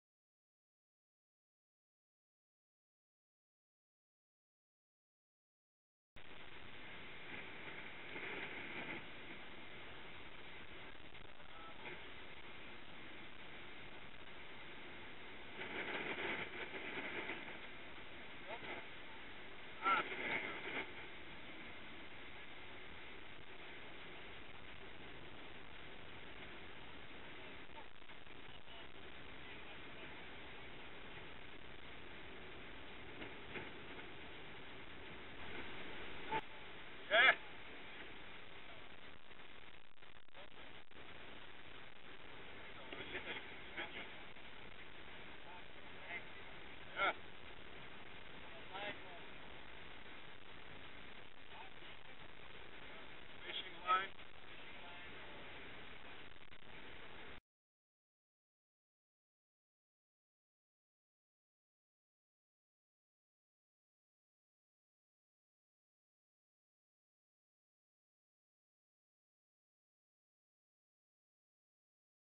Bounty Adventures Curacao | Pilot Whales
Pilot Whales swimming with the Jonalisa! (3 december 2011)